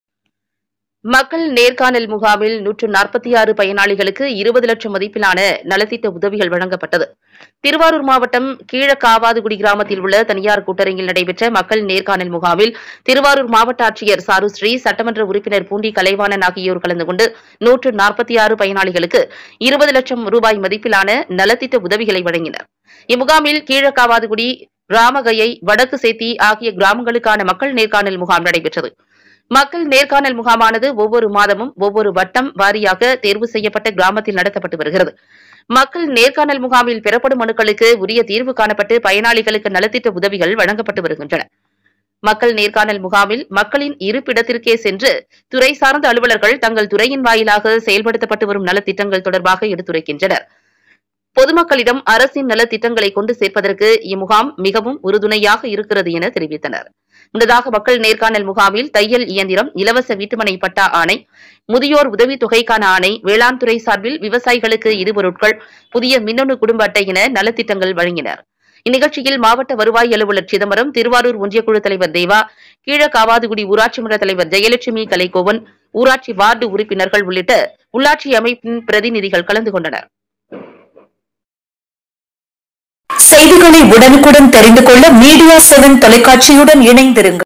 திருவாரூர் மாவட்ட ஆட்சியருடன் மக்கள் நேர்காணல் முகாம்...
திருவாரூர்மாவட்டம்  கிழக்காவாதுக்குடி கிராமத்தில் உள்ள தனியார் கூட்டரங்கில்  நடைபெற்ற மக்கள் நேர்காணல் முகாமில்  திருவாரூர் மாவட்டஆட்சியர் சாருஸ்ரீ  , சட்டமன்ற உறுப்பினர் பூண்டிகலைவாணன்  கலந்துகொண்டு  146 பயனாளிகளுக்கு ரூ.20 இலட்சம் மதிப்பிலான நலத்திட்ட உதவிகளை  வழங்கினர் ,

இம்முகாமில், கீழக்காவாதுக்குடி, இராமகயை கிராமம், வடக்குசேத்தி ஆகிய கிராமங்களுக்கான மக்கள் நேர்காணல் முகாம்  நடைபெற்றது.

மக்கள் நேர்காணல் முகாமானது, ஒவ்வொரு மாதமும், ஒவ்வொரு வட்டம் வாரியாக, தேர்வு செய்யப்பட்ட கிராமத்தில் நடத்தப்பட்டு வருகிறது , மக்கள் நேர்காணல் முகாமில் பெறப்படும் மனுக்களுக்கு உரியதீர்வு காணப்பட்டு பயனாளிகளுக்கு நலத்திட்ட உதவிகள் வழங்கப்பட்டு வருகிறது , மக்கள் நேர்காணல் முகாமில் மக்களின் இருப்பிடத்திற்கே சென்று துறைச்சார்ந்த அலுவலர்கள் தங்கள் துறையின் வாயிலாக செயல்படுத்தப்பட்டுவரும் நலத்திட்டங்கள் தொடர்பாக எடுத்துரைக்கின்றனர் , பொதுமக்களிடம் அரசின் நலதிட்டங்களை கொண்டு சேர்ப்பதற்கு இம்முகாம் மிகவும் உறுதுணையாக இருக்கிறது  என தெரிவித்தனர் ,

முன்னதாக, மக்கள் நேர்காணல் முகாமில்  தையல்எந்திரம் , இலவச வீட்டு மனை பட்டா ஆணை , முதியோர் உதவி தொகைக்கான ஆணை , வேளாண் துறை சார்பில் விவசாயிகளுக்கு  இடுபொருள்கள் , புதிய மின்னணு குடும்ப அட்டை  என நலத்திட்டங்கள் வழங்கினர் ,

இந்நிகழ்ச்சியில், மாவட்டவருவாய் அலுவலர் சிதம்பரம், திருவாரூர் ஒன்றியக்குழு தலைவர் தேவா , கீழக்காவாதுக்குடி ஊராட்சி மன்ற தலைவர் ஜெயலெட்சுமிகலைகோவன் , ஊராட்சி வார்டு உறுப்பினர்கள் ,  உள்ளிட்ட உள்ளாட்சி அமைப்பின் பிரதிநிதிகள் கலந்துகொண்டனர்.